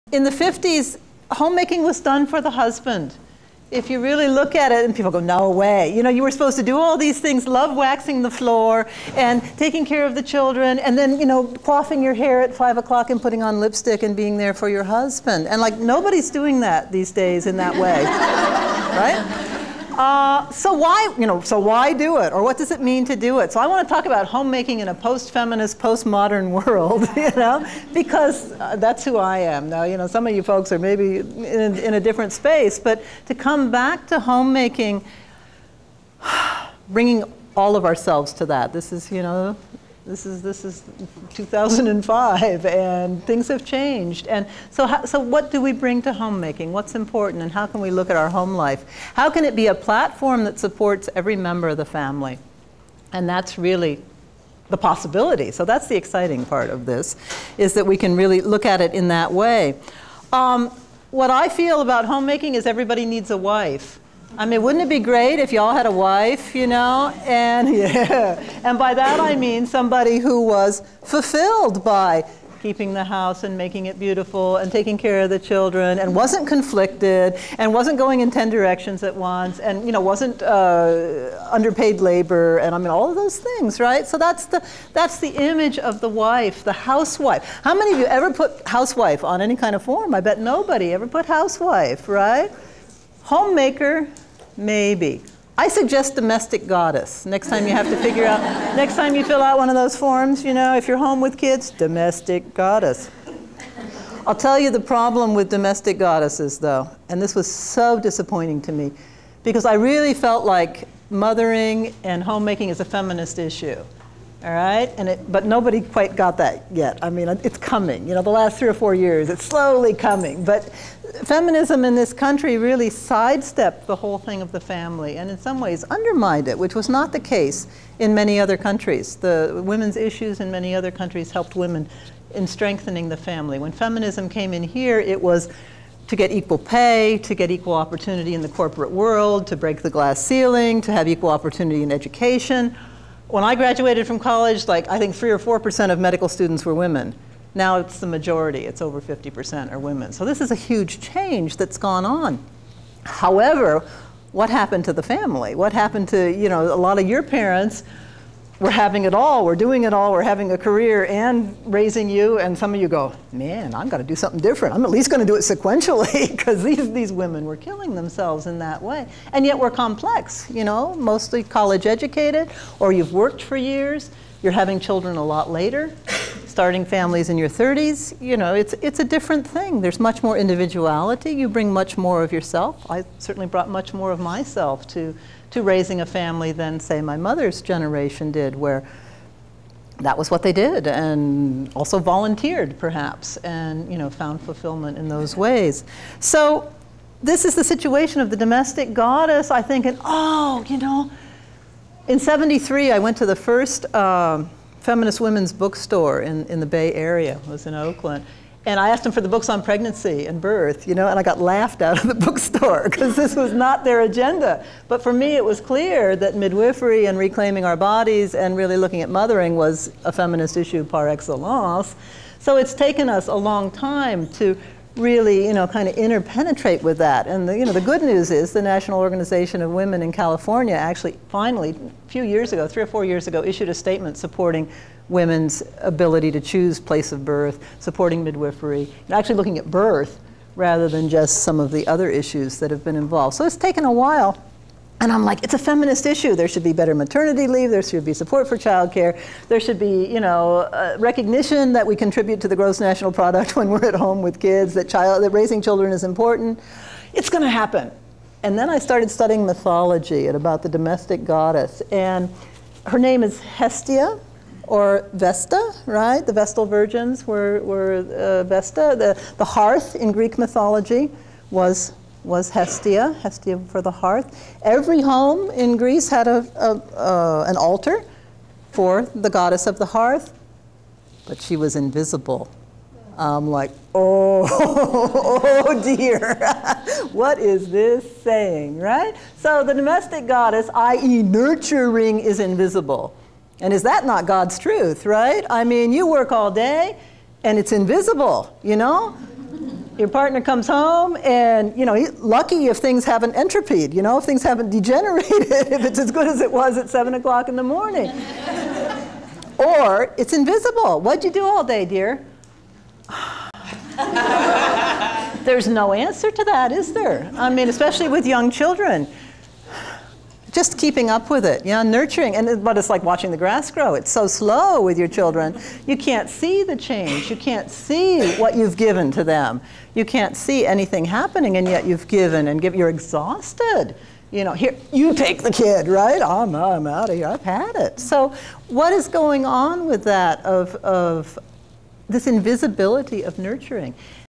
Your Are Your Childs First Teacher by Rahima Baldwin by
Get a free copy of this book and video when you purchase the Earthschooling preschool program

This is a sample clip from the Waldorf in the Home Conference from a speech given by Rahima Baldwin called, "Family Matters". Rahima is the author of the widely popular and classic book "You Are Your Child's First Teacher". You can order the entire DVD and more than 200 other titles If you would like to see some more Waldorf videos (including full legnth videos!)